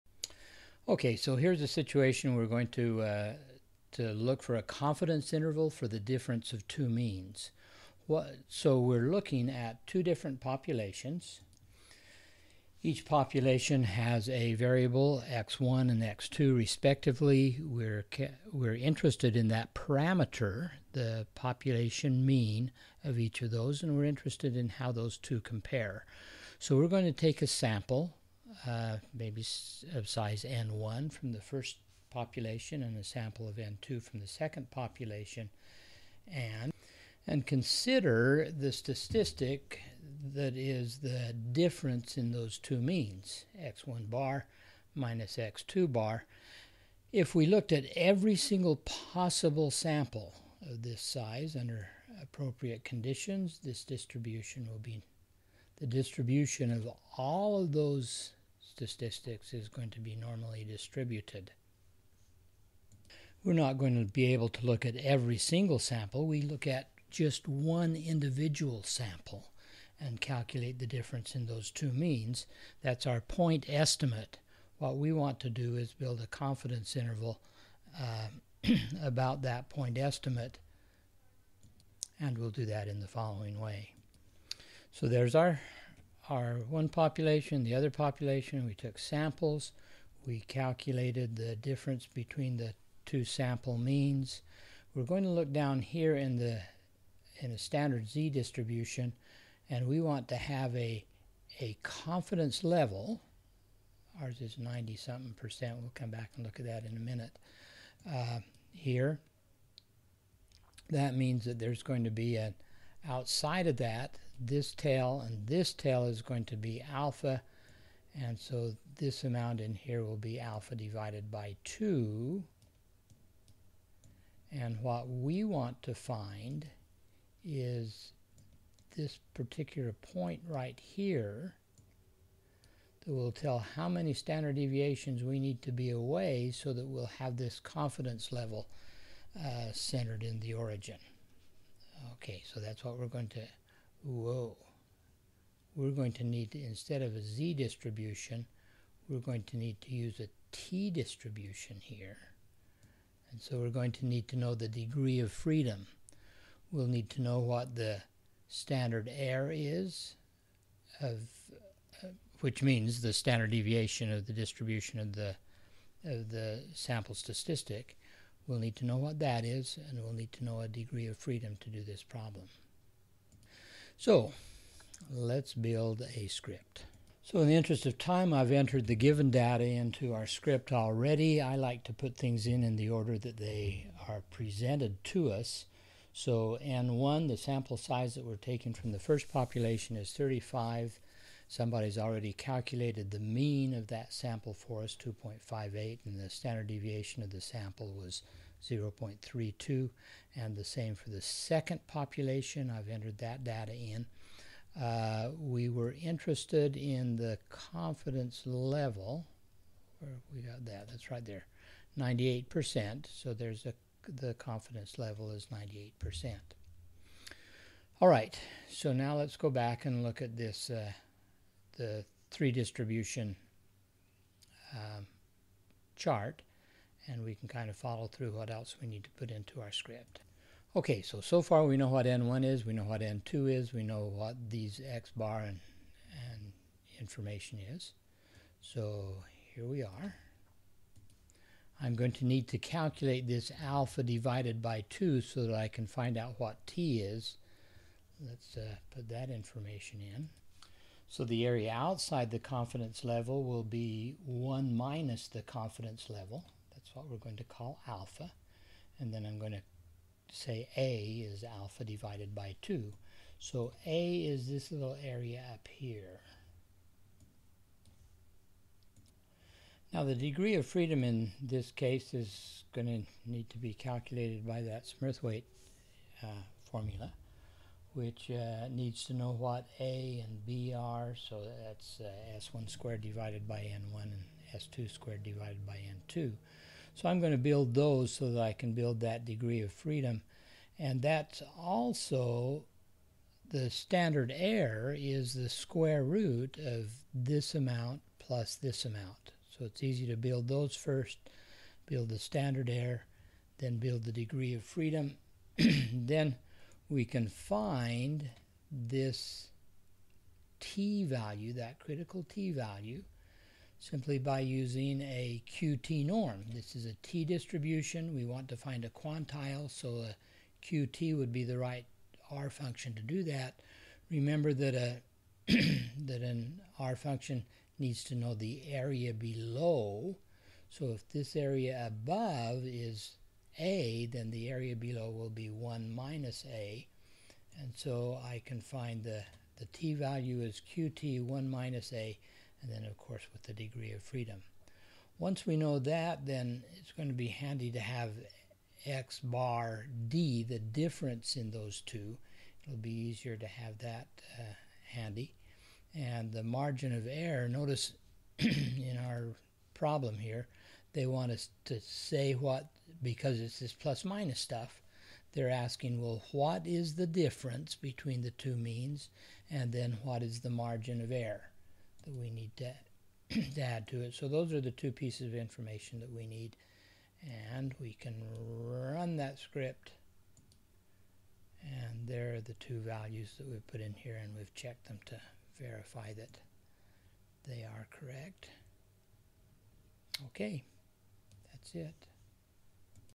Q2chp9MOM
Confidence interval for the difference of two independent means is calculated.